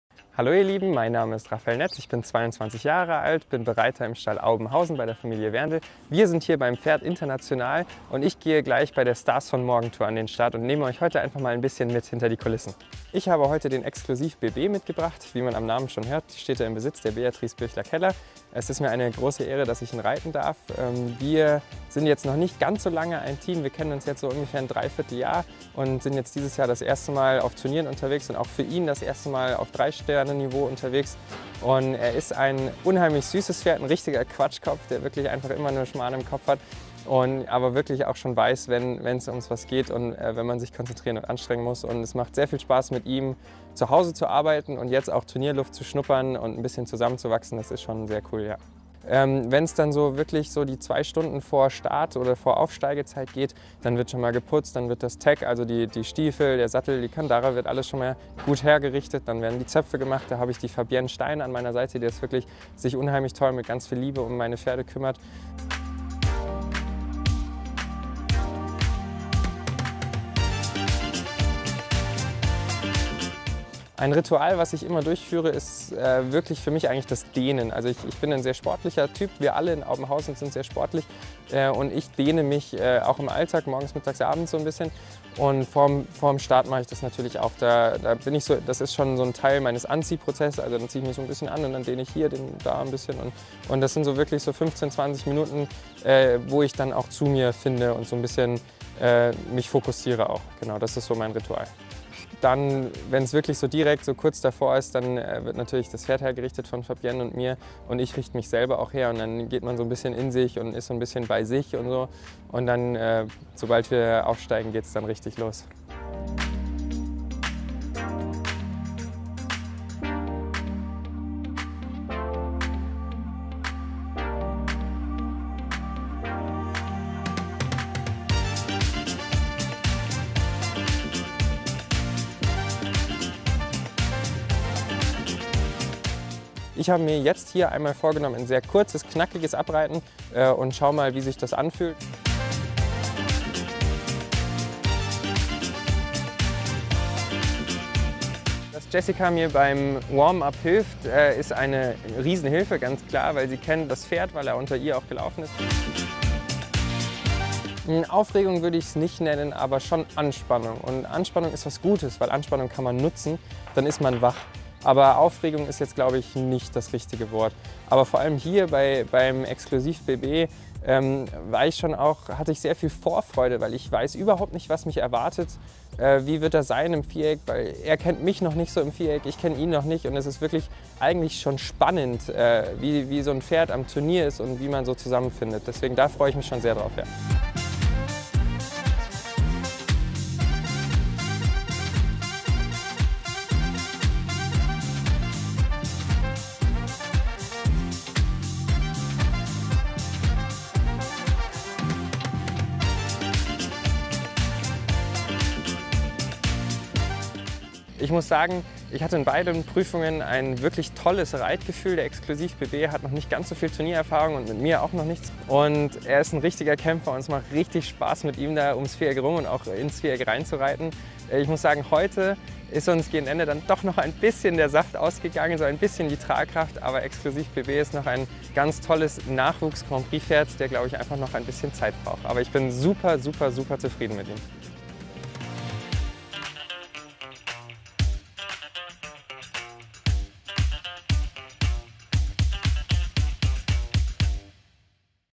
FMA Raphael Netz Pferd International
Raphael Netz ist auf jeden Fall einer der "Stars von Morgen", denn am vergangenen Wochenende konnte er beide Prüfungen der Serie mit Exclusive BB für sich entscheiden. Gemeinsam mit ClipMyHorse nahm er euch mit hinter die Kulissen, zeigte euch die Abläufe vor seinem Start und erzählte von seinem Verhältnis zu seinem Pferd Exclusive BB.